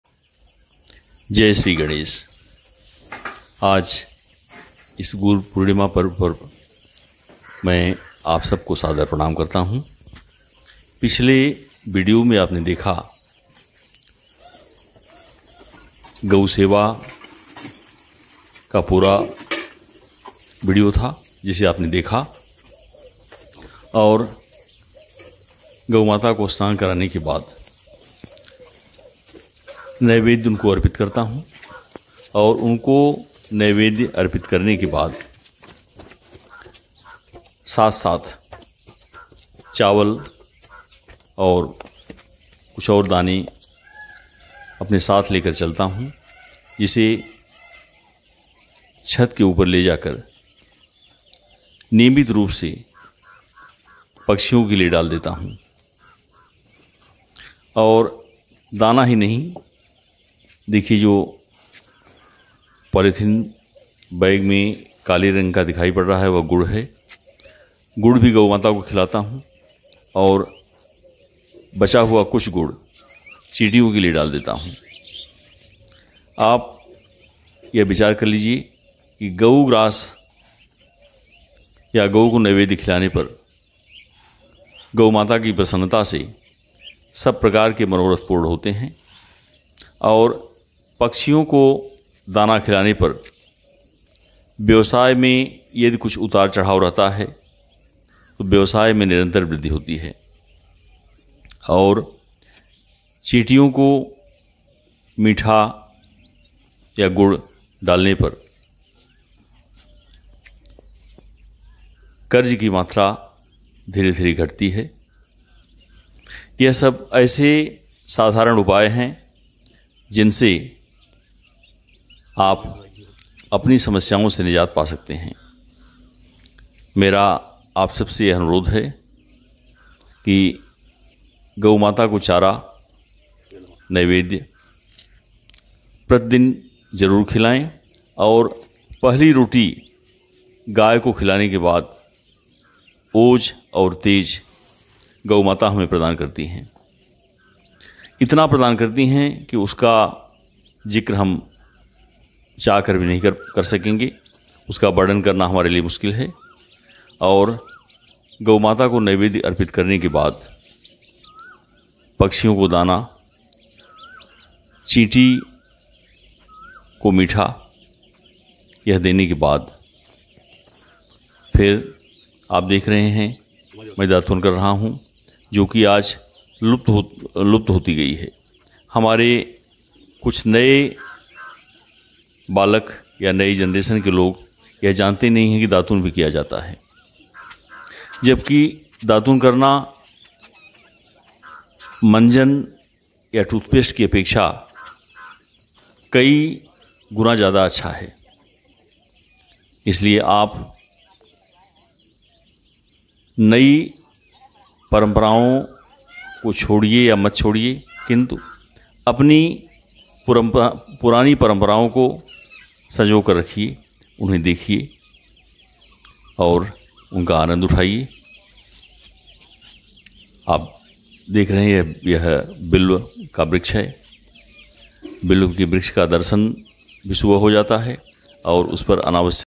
go seva#गौ माता की सेवाचिड़ियों को दानाचिंटे को गुण#
गऊ माता की सेवा, चिड़ियों को दाना और चीटियों को गुड़ खिलाने के लाभ